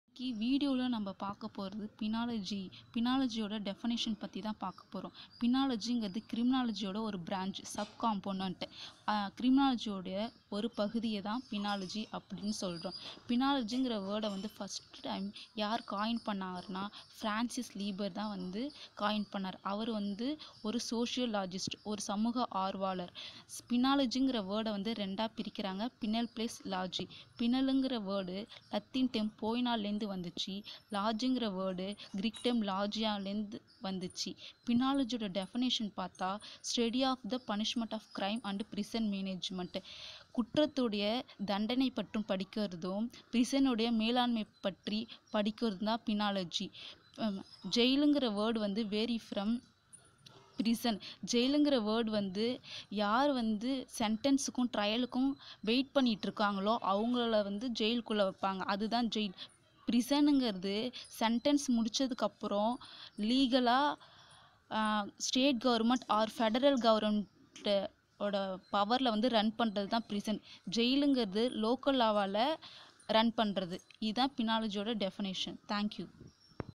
Penology definition in Tamil
Penology
(from "penal", Latin poena, "punishment"
and the Greek suffix -logia, "study of")
is a sub-component of criminology.
Penology - the study of the punishment of crime and of prison management.